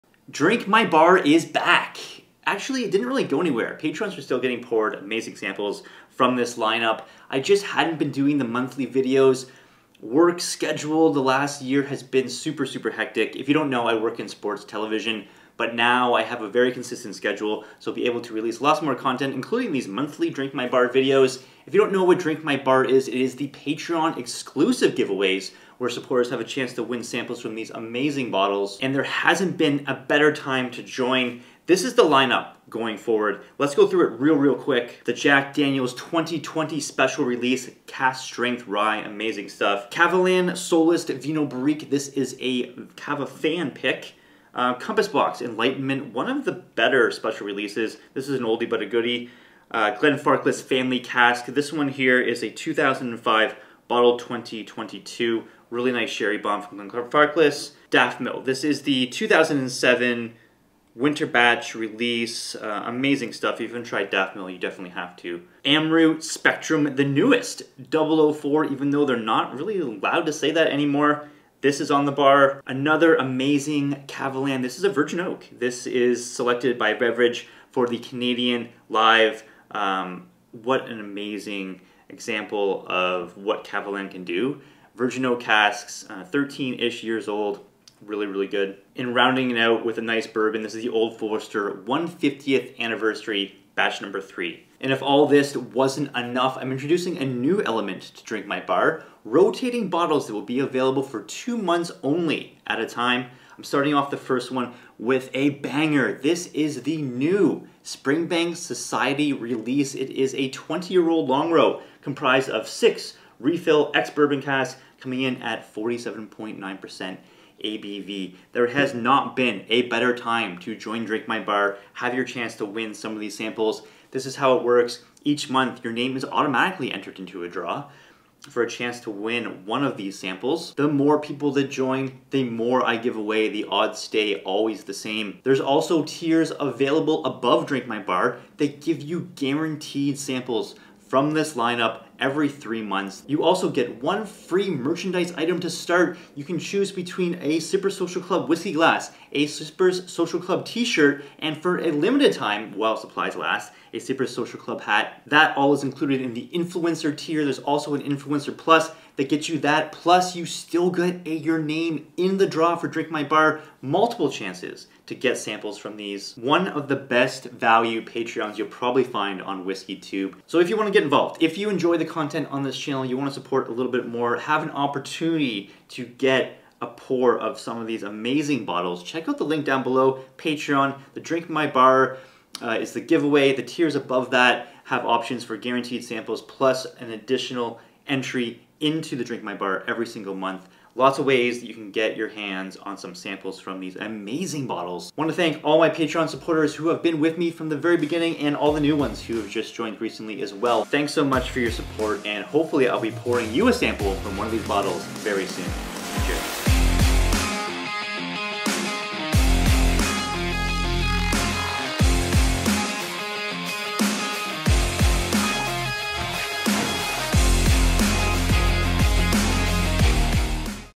DRINK MY BAR IS BACK!
Monthly Drink My Bar videos are back, and the options are stacked! Leave a comment: Which bottle excites you most?

Jack Daniels 2020 Special Release - Single Barrel, Barrel Proof Rye
Kavalan Solist Vinho Barrique - KavaFan Selection
Compass Box - Enlightenment
Glenfarclas Family Cask 2005
Daftmill 2007 Winter Batch
Amrut Spectrum 2022
Kavalan Solist Virgin Oak - Bevridge Selection
Old Forester 150th Anniversary Batch 3
Springbank Society Release - Longrow 20 Year Old